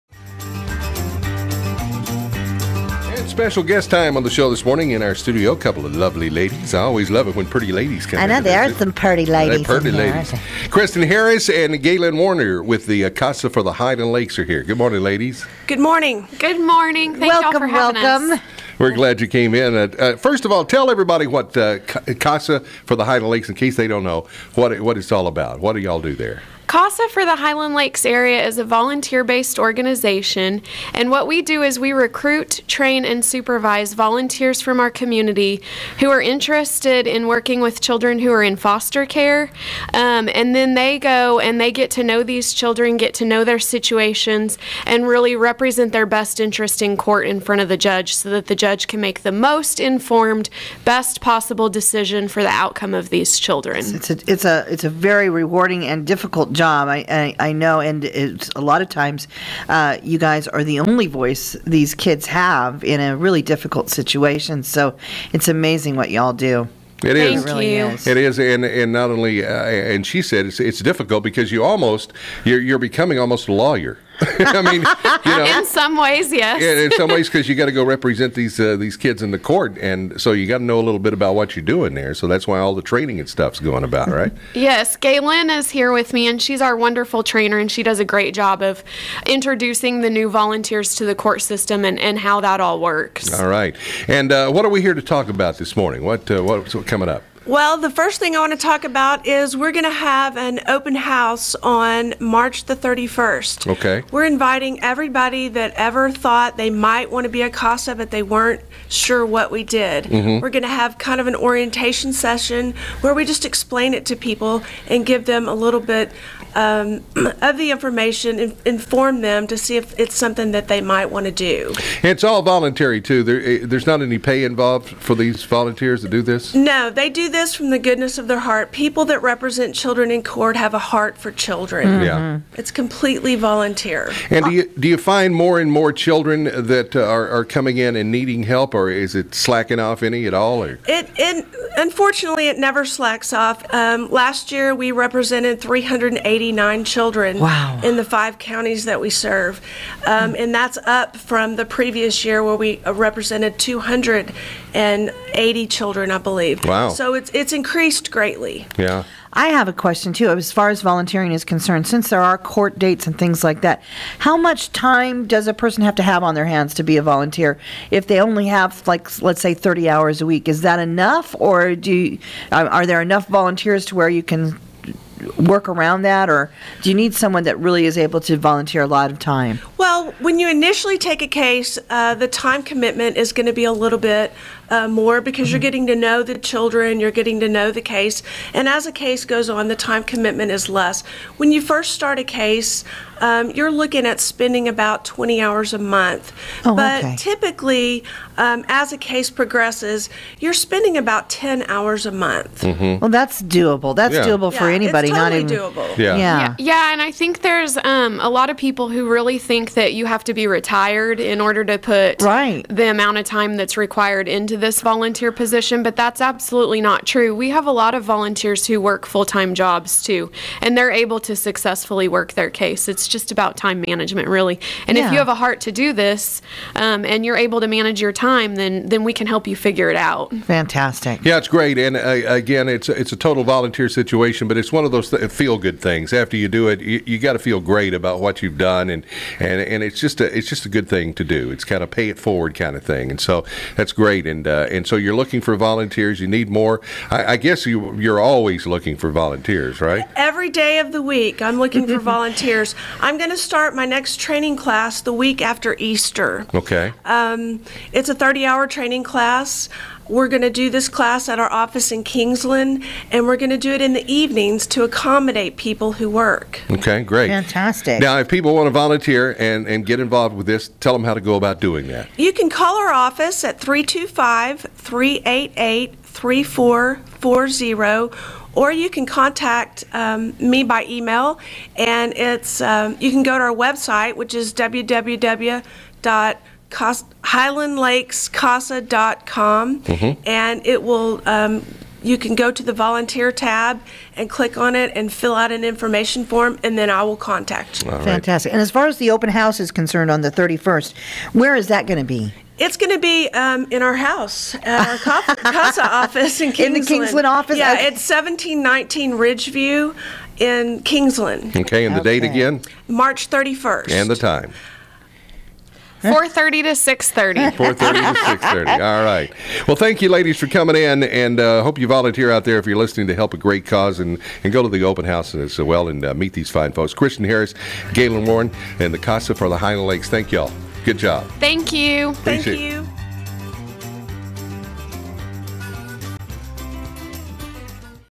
Highland Lakes CASA open house | KBEY 103.9 FM
A big thanks to Galyn Woerner and Kristen Harris for visiting with us this morning about all the wonderful things CASA does!